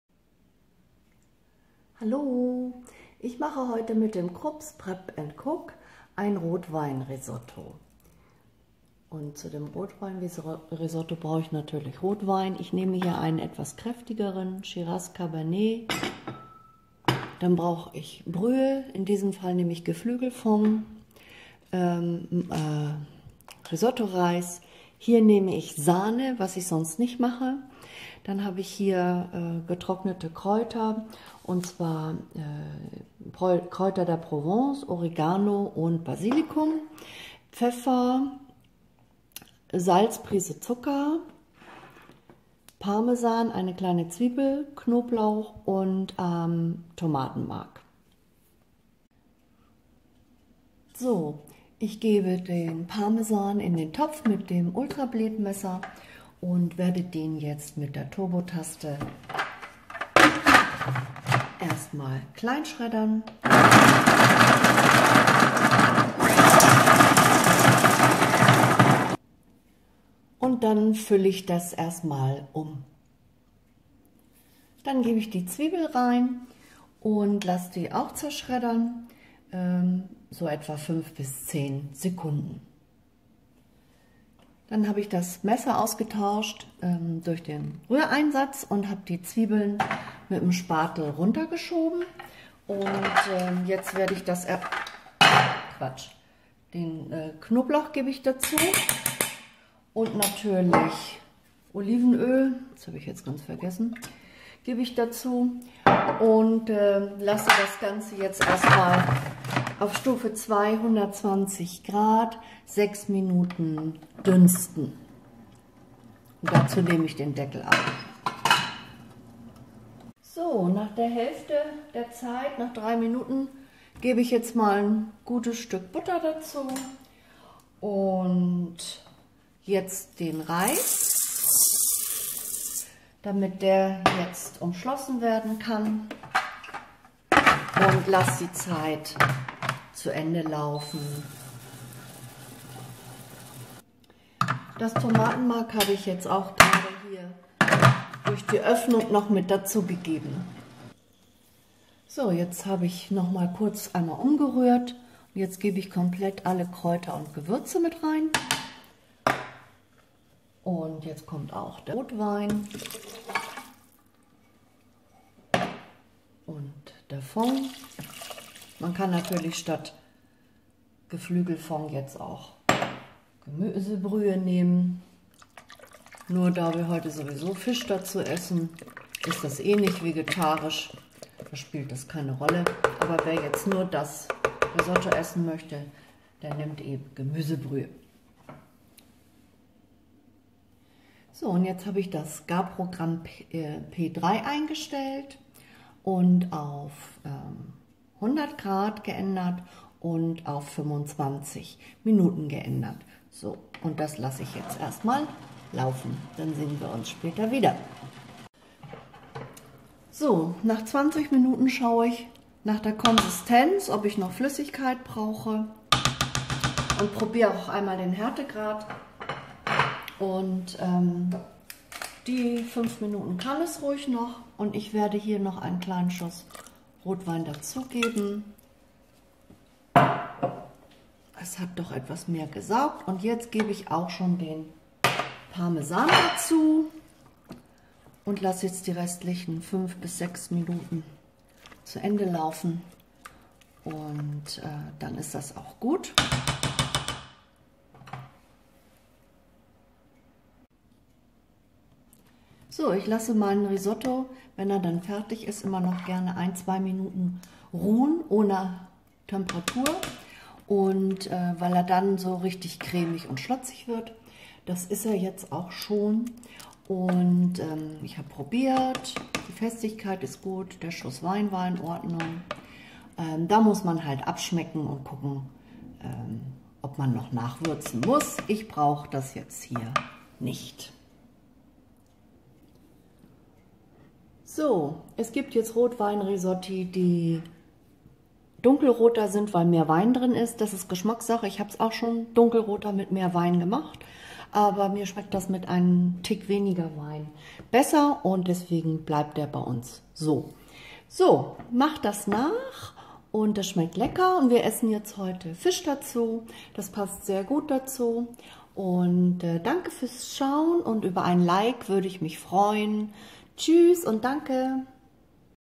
Krups Prep & Cook ▪ Rotwein Risotto •milder Weingeschmack•cremig•lecker•
Kochen  mit Krups Prep & Cook

Rotwein Risotto

220 g Risotto Reis
400 ml Hühnerfond
200-250 ml kräftiger Rotwein
200 g Sahne

Olivenöl und Butter
1 kleine Zwiebel und 1 Knoblauchzehe
1 EL Tomatenmark
je 1 EL getrockn. Kräuter: Oregano, Basilikum, Kräuter der Provence
Pfeffer, Prise Zucker, evtl. Prise Salz (später abschmecken)

60 - 80 g geriebener Parmesan